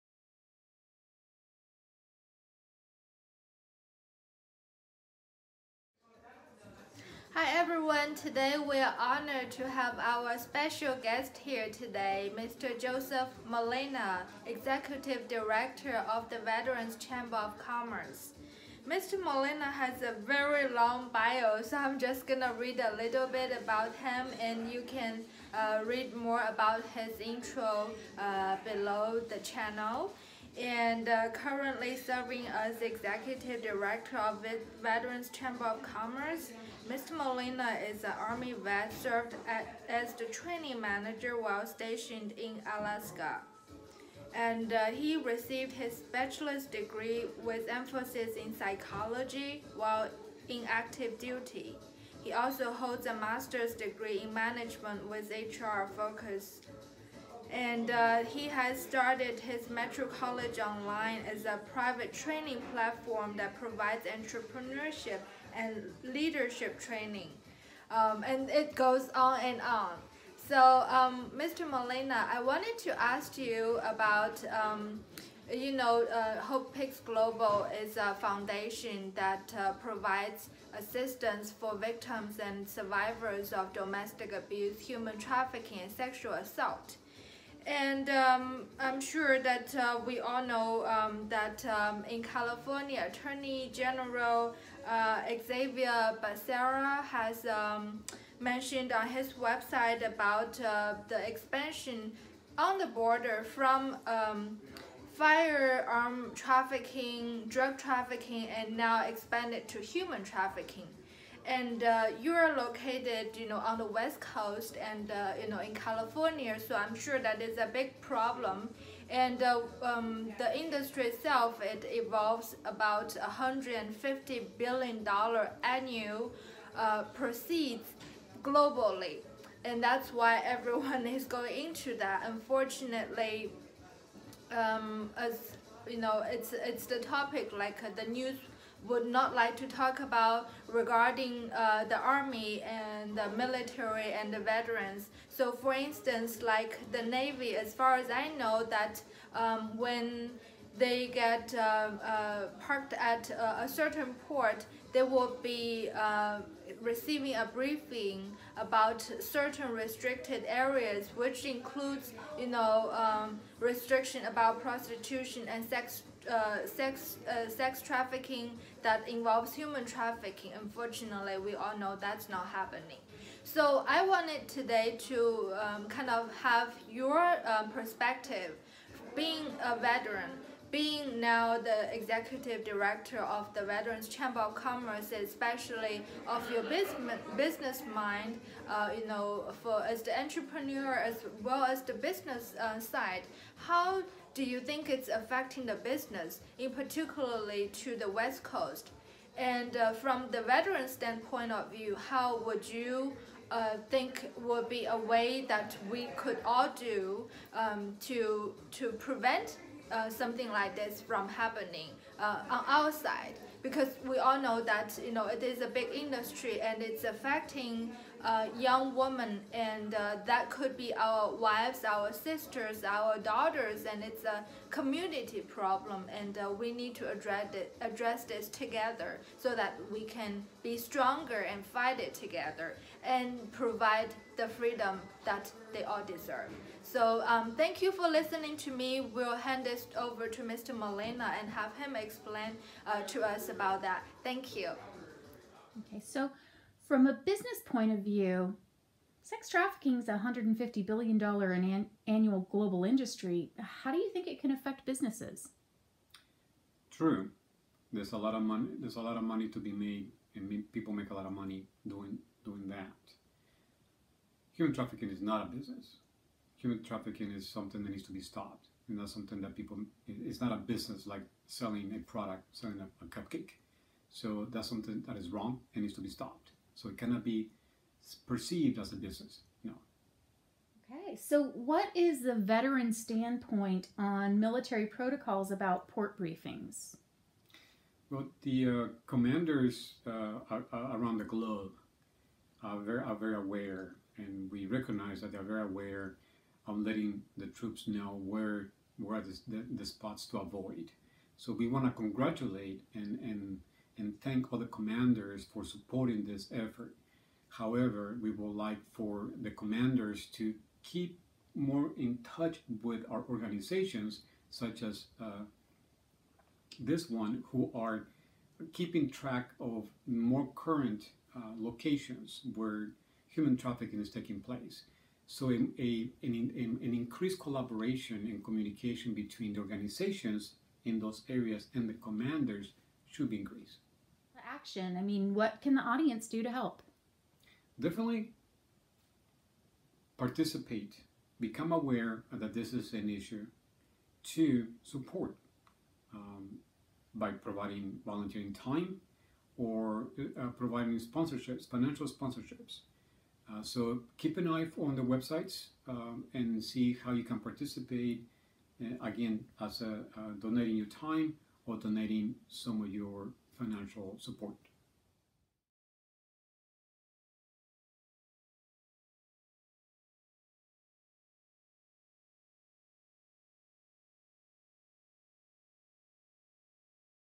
Veterans on Human Trafficking Awareness
January is Human Trafficking Awareness Month. According to UNICEF USA, the 2009 U.S. State Department Trafficking in Persons Report cited a London-based study, which found that almost 70% of adult female trafficking victims experienced domestic violence prior to being trafficked.
The victims of abuse can’t leave if they don’t have a place to go. Hope Pyx Global is that place dedicated to helping victims and survivors of domestic abuse from all backgrounds.

Joseph Molina’s Bio
National Veterans Chamber of Commerce:
Currently serving as the Executive Director for the Veterans Chamber of Commerce. Mr. Molina is an Army Vet. Served as the Training Manager while stationed in Alaska. Received a BA Degree with emphasis in Psychology while in Active Duty.  He holds and a Master’s degree in Management with HR focus. Mr Molina started MetroCollegeOnline a Private Training platform that provides Entrepreneurship and Leadership training.
Co-Founder of Alliance Funding Group, a Micro-enterprise that supports microbusinesses with access to capital. Mr. Molina has over 12 years’ experience as a business advisor with extensive experience in business concepts, strategies and employee leadership.
Faculty member since 1998 for several Universities and Community Colleges. Courses on; Management for Park University and lead instructor for the Leadership Academy at MiraCosta College, Veterans in Agriculture at Cal Poly Pomona and Marketing for Entrepreneurs at Cerritos College.

Highlights include:
1. Implemented the “Meet the Buyers” sessions. Under his leadership, the program grew from 40 attendees to over 200 small business owners.
2. Developed specialized curriculum for veterans to help transition to small business ownership and veteran management training.
3. Developed partnerships with Small Business Development Centers, PTAC, Chambers of Commerce and Veteran Business Centers.
4. Has been helping Colleges collaborate with military bases.
5. Provide Consultation and Advice to Faculty on how teaching Entrepreneurship.
6. Author of Internships for Veteran Entrepreneurs, a program that awards Academic credit for students starting a business.

Developed the following programs:
1. The program allows for community colleges to award “Academic Credit” to students while working on their own business.
2. Created Textbook Companion (Workbook) - to help support faculty on implementing Entrepreneurial activities and real life applications in the classroom.
3. Soft Skills Pro -- a program that trains Teachers Faculty and Counselors on “how to” teach students Job readiness skills to be successful in a job.
5. Textbook Author, “Management Guide to Creating High Performance Employees”
6. Military – “Military to Civilian Re-Adjustment Program” (M-CRAP) To help transitioning military re-enter into the civilian market at a Supervisory Levels.